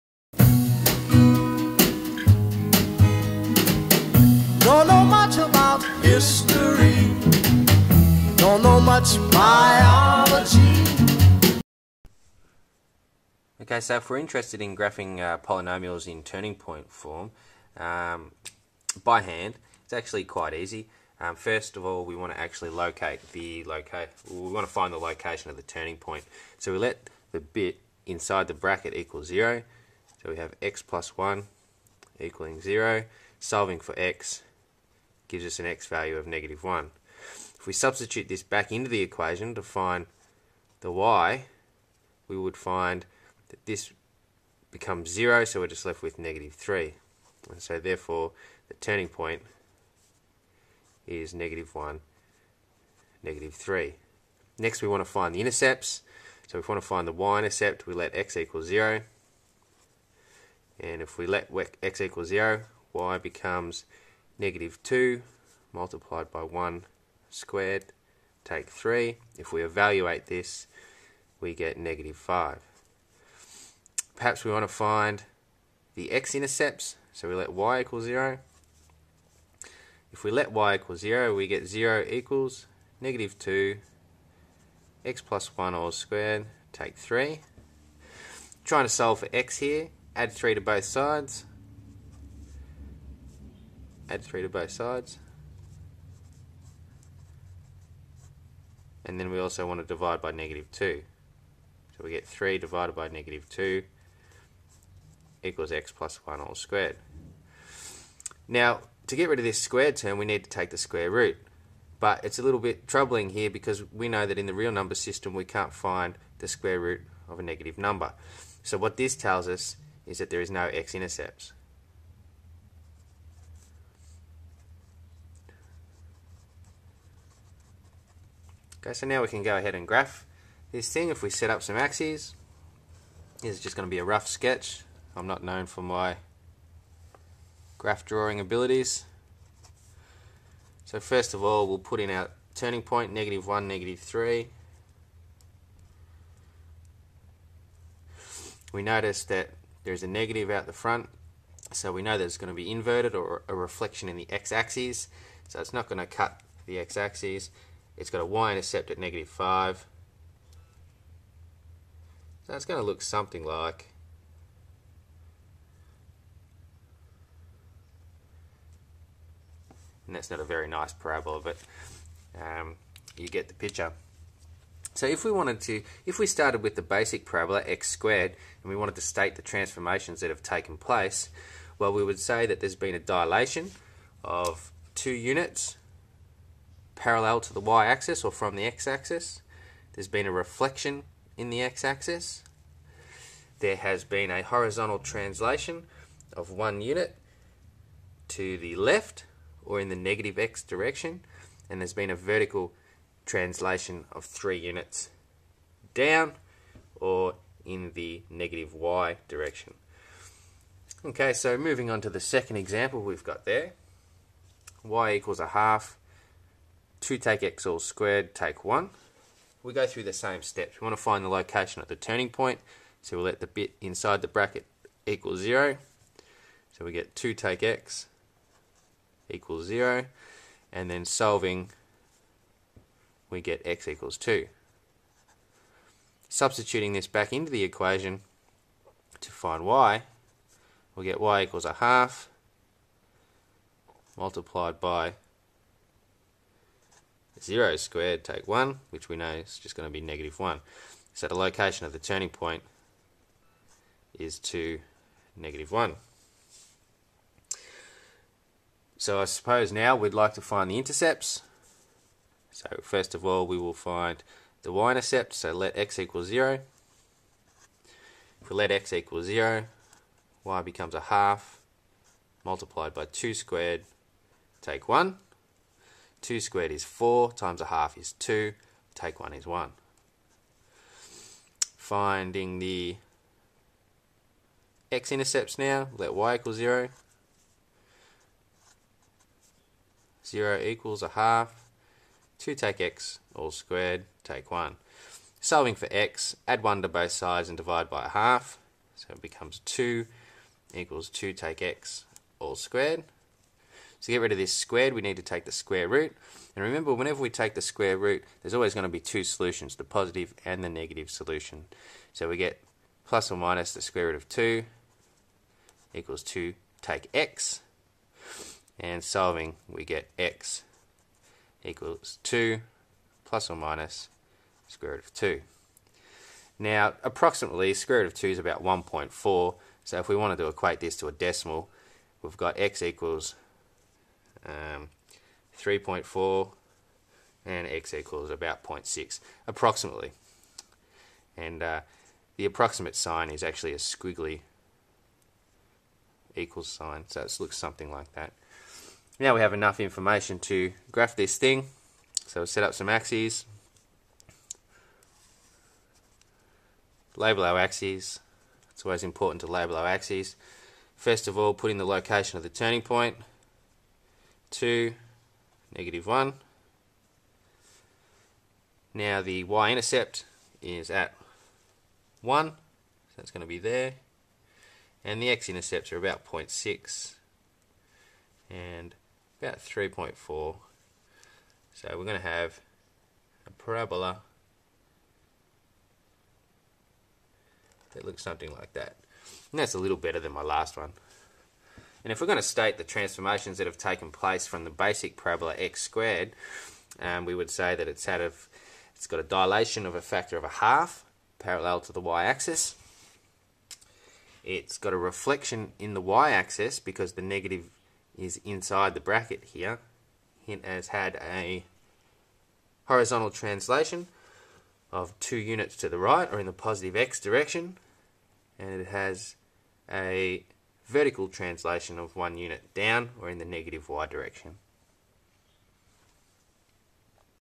Graphing Quadratics: Turning Point
Two examples of graphing quadratics in turning point form.